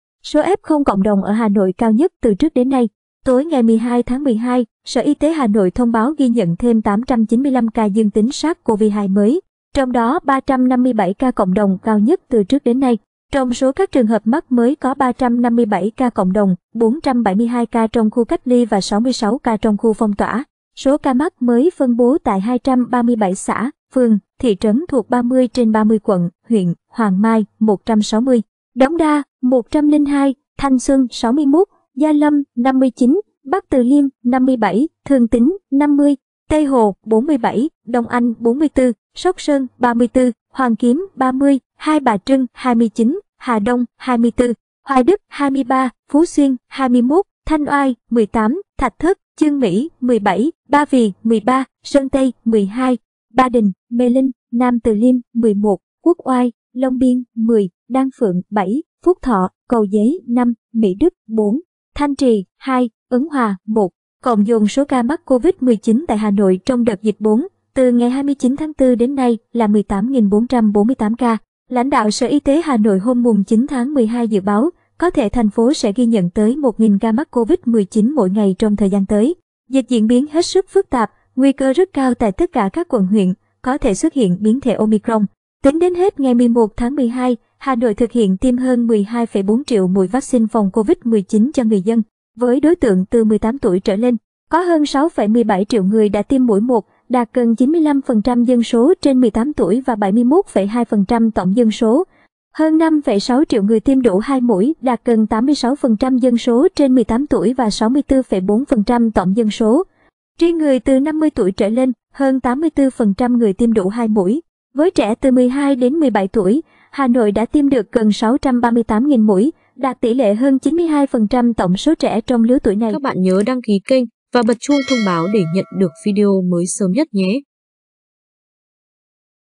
Số F0 cộng đồng ở Hà Nội cao nhất từ trước đến nay
Tối 12/12, Sở Y tế Hà Nội thông báo ghi nhận thêm 895 ca dương tính SARS-CoV-2 mới, trong đó 357 ca cộng đồng - cao nhất từ trước đến nay.Riêng người từ 50 tuổi trở lên, hơn 84% người tiêm đủ 2 mũi. Với trẻ từ 12 - 17 tuổi, Hà Nội đã tiêm được gần 638.000 mũi, đạt tỷ lệ hơn 92% tổng số trẻ trong lứa tuổi này.
Lãnh đạo Sở Y tế Hà Nội hôm 9/12 dự báo, có thể thành phố sẽ ghi nhận tới 1.000 ca mắc COVID-19 mỗi ngày trong thời gian tới, dịch diễn biến hết sức phức tạp, nguy cơ rất cao tại tất cả các quận huyện, có thể xuất hiện biến thể Omicron.
Cộng dồn số ca mắc COVID-19 tại Hà Nội trong đợt dịch 4 (từ ngày 29/4 đến nay) là 18.448 ca.
Biểu đồ ca mắc của Hà Nội từ ngày 10/11 đến 12/12.
Trong số các trường hợp mắc mới có 357 ca cộng đồng, 472 ca trong khu cách ly và 66 ca trong khu phong tỏa.
Tính đến hết ngày 11/12, Hà Nội thực hiện tiêm hơn 12,4 triệu mũi vaccine phòng COVID-19 cho người dân. Với đối tượng từ 18 tuổi trở lên, có hơn 6,17 triệu người đã tiêm mũi 1 (đạt gần 95% dân số trên 18 tuổi và 71,2% tổng dân số); hơn 5,6 triệu người tiêm đủ 2 mũi (đạt gần 86% dân số trên 18 tuổi và 64,4% tổng dân số).